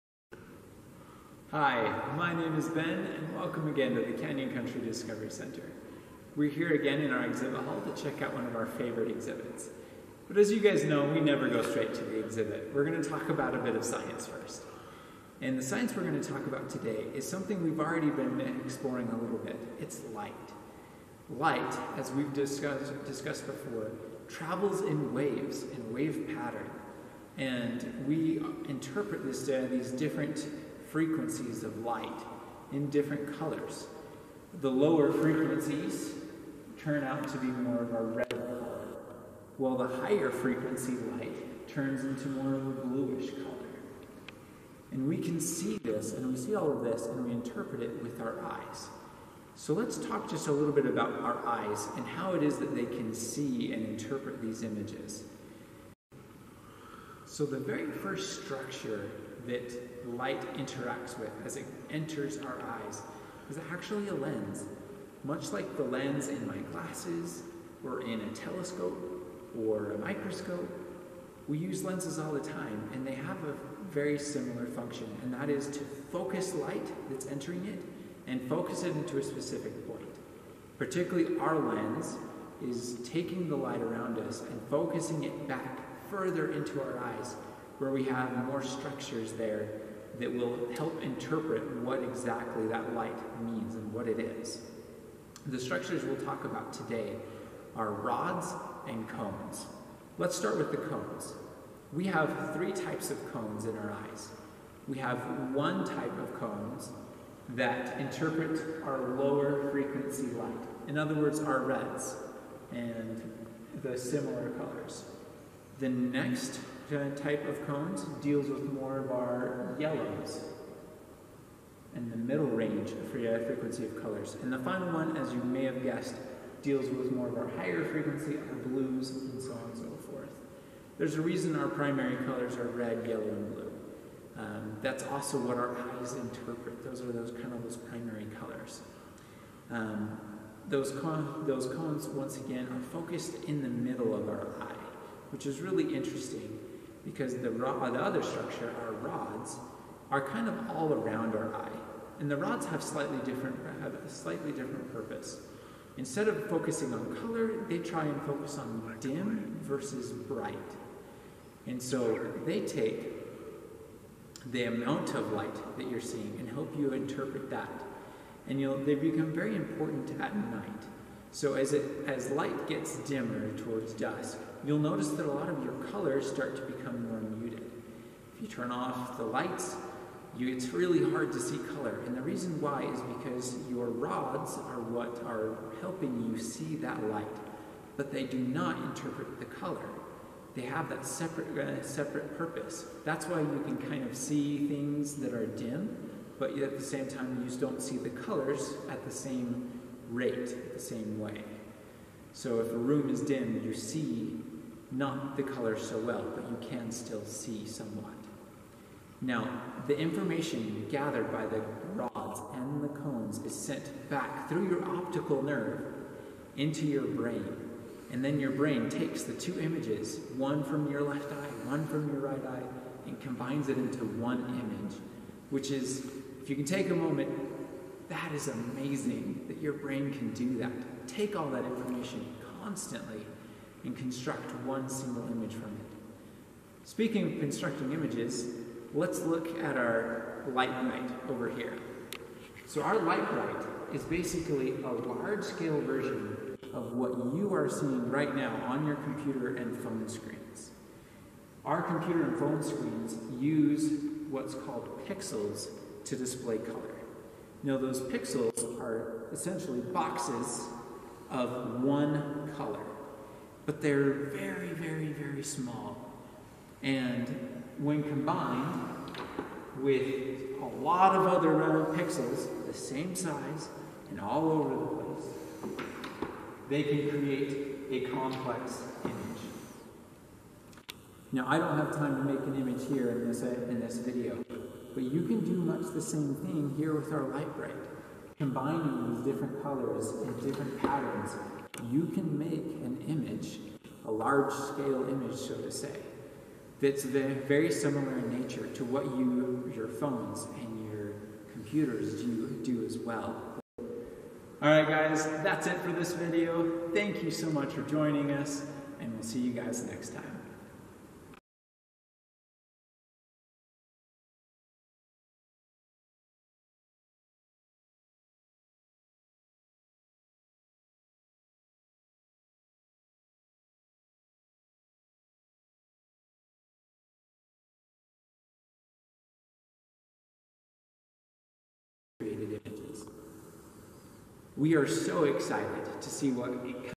Virtual Field Trip- Light Bright and Vision
In this video, Ben explains the difference between Cone and Rod cells in the eye, how we see color, and how pixels work to form images. All with the use of our Giant Light Bright!